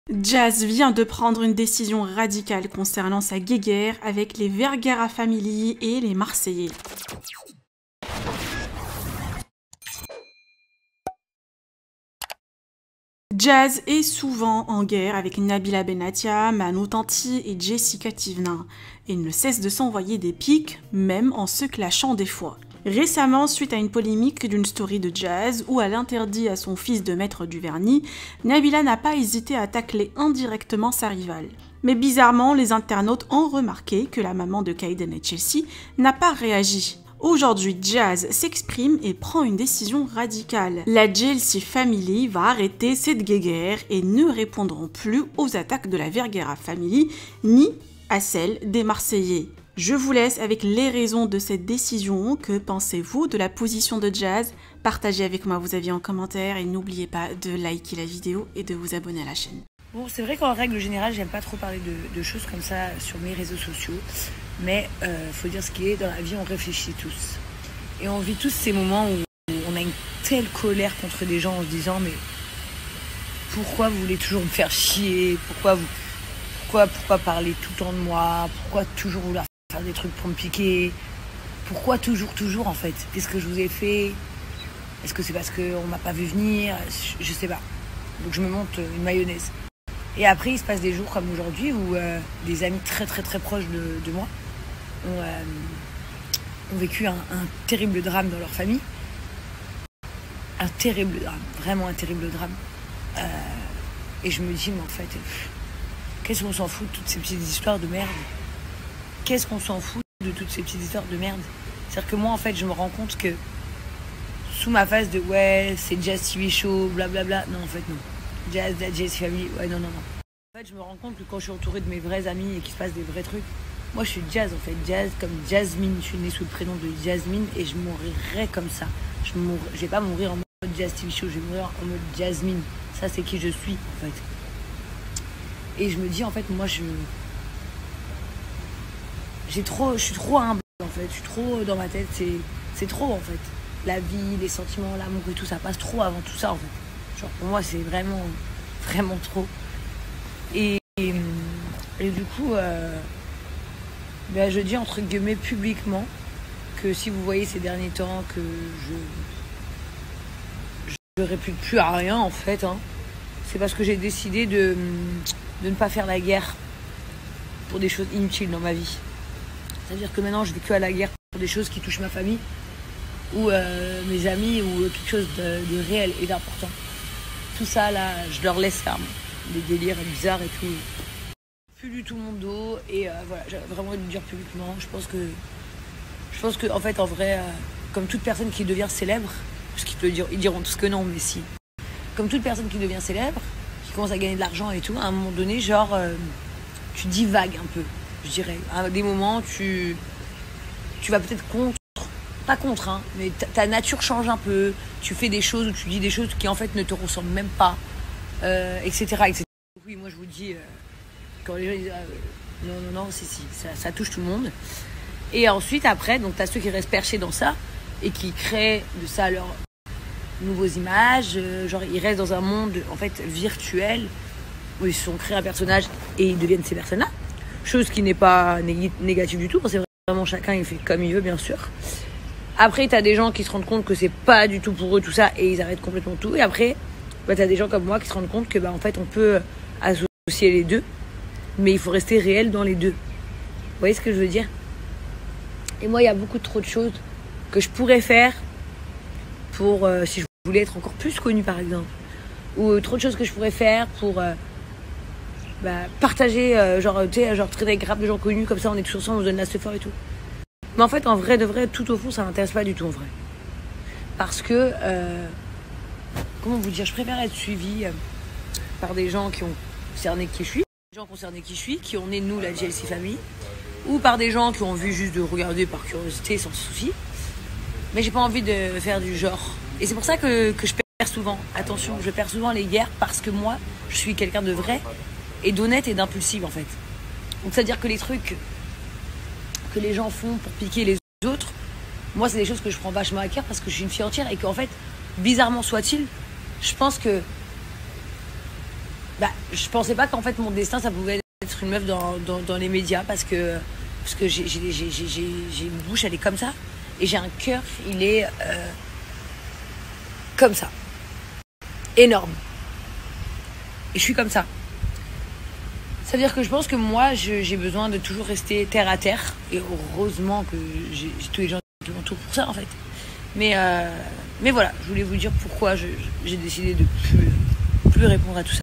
JAZZ PREND UNE DÉCISION RADICALE APRÈS LES PICS DE NABILLA ET SA GUEGUE*RRE AVEC LES MARSEILLAIS
Jazz s'envoie souvent des pics avec Nabilla Benattia et Jessica Thivenin. Une rivalité qui dure depuis un moment!
Récemment, Nabilla a tac*lé indirectement Jazz suite à sa polé*mique sur le vernis à ongle pour son fils Cayden. La maman de ce dernier n'a pas réagi, a enfin décidé ses raisons.
Pourquoi Jazz décide d'arrêter de répondre aux marseillais, et à la  Vergara Family? Les raisons enfin dévoilées!